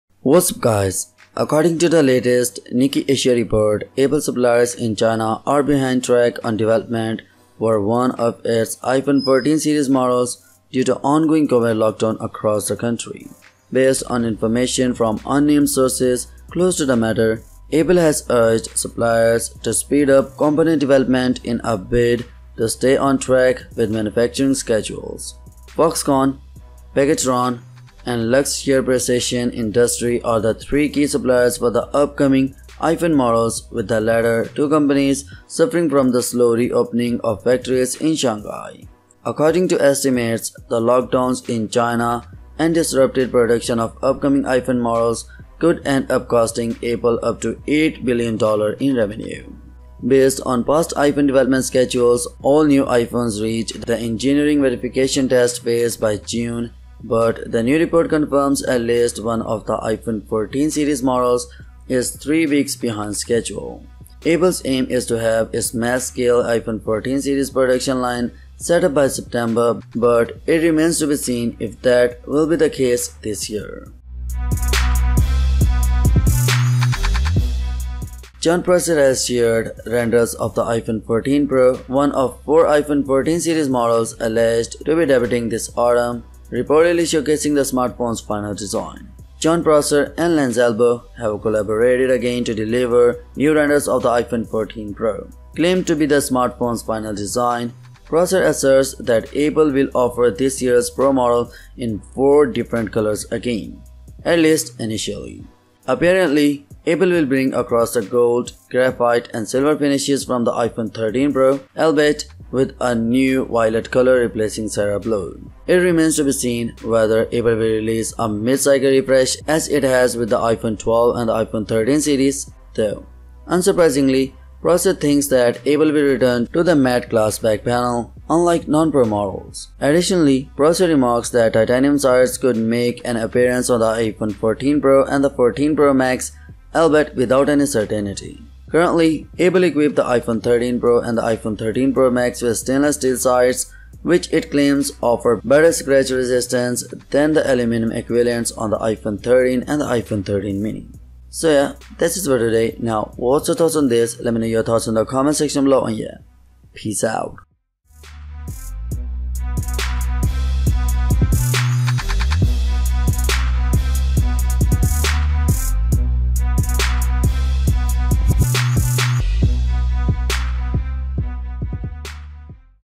Apple iPhone 14 Pro – A BIG NEWS & FIRST LOOK!
Finally, Jon Prosser and Ian Zelbo have collaborated again to deliver new renders of the iPhone 14 Pro. Jon also Claimed to be the final design of the iPhone 14 Pro... also we have a BAD or we can say SAD news for all the iPhone Lovers...iPhone 14 Series may be delayed due to Lockdown in china.

PEACE!!